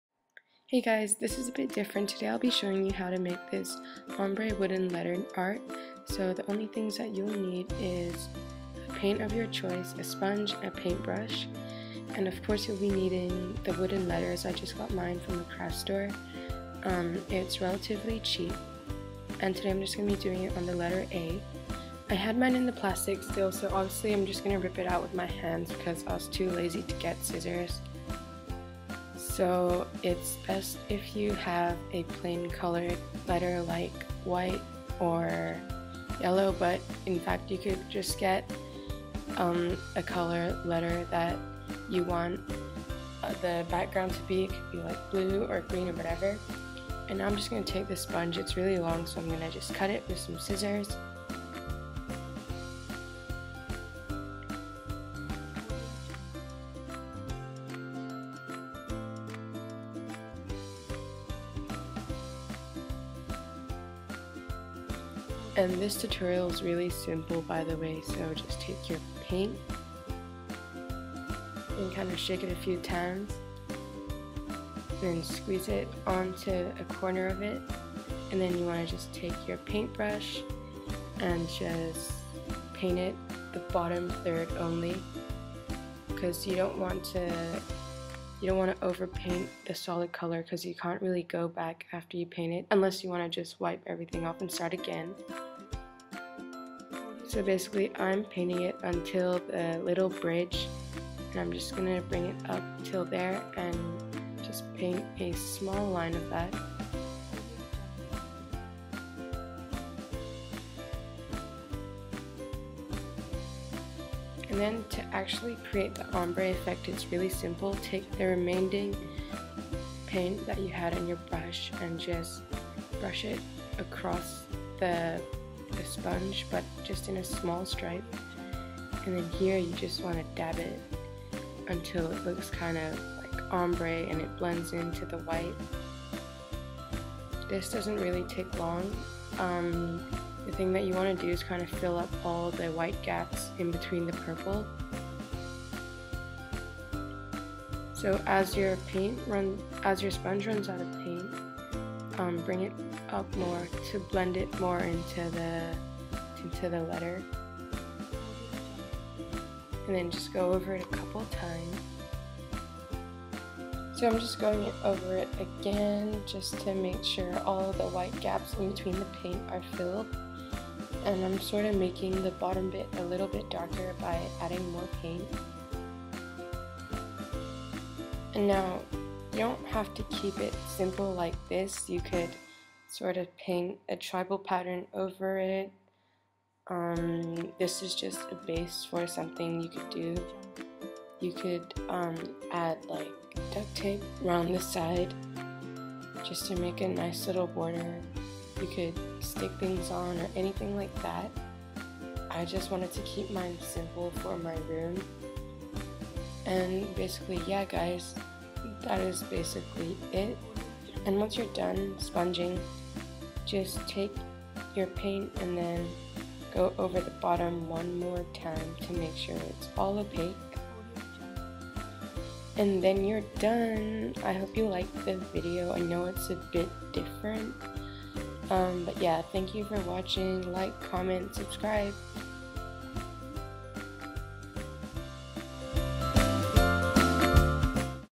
DIY - EASY OMBRE WOODEN LETTER
Hello! This is a bit different but I decided to try something with the new wooden letters that I received as a gift! This is super simple and easy all you need it a paintbrush, sponge, paint! Bye!

Thanks! {◕ ◡ ◕}